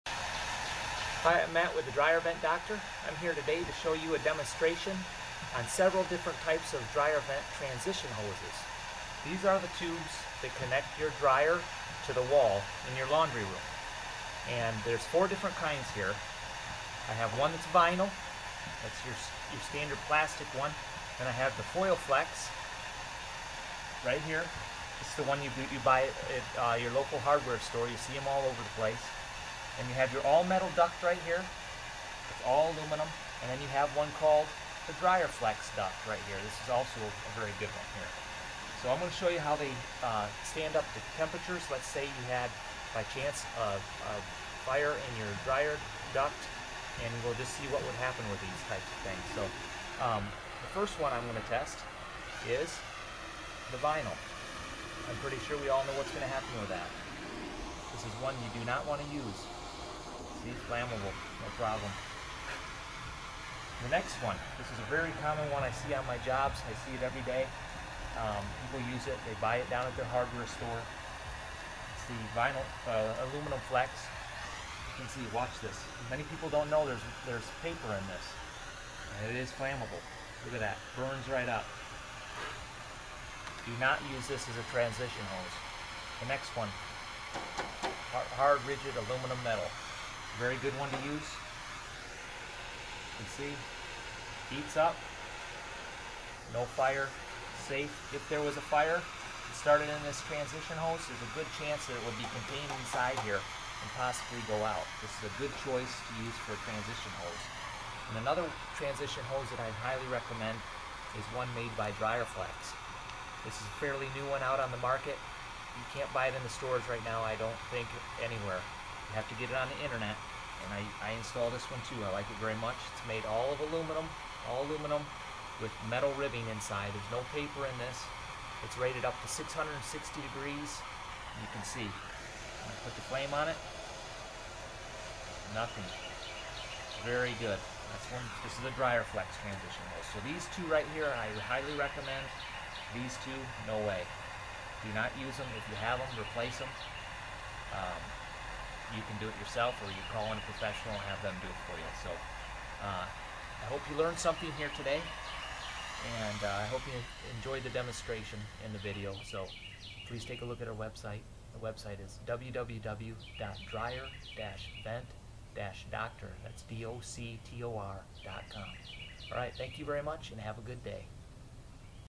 Dryer vent tube fire safety test.
How safe is your dryer vent? Matt with the Dryer Vent Doctor tests how flammable common dryer vent transition tubes are. We tested plastic, foil, and semi rigid metal. You'll be amazed how fast you can have a dryer vent fire on your hands. Dryer Vent Doctor is spreading the word about the dangers of a clogged or crushed dryer vent transitions hoses.  Make sure you only use fully metal transition hoses on your dryers.  Foil flex and plastic flex hoses are fire hazards and should not be used.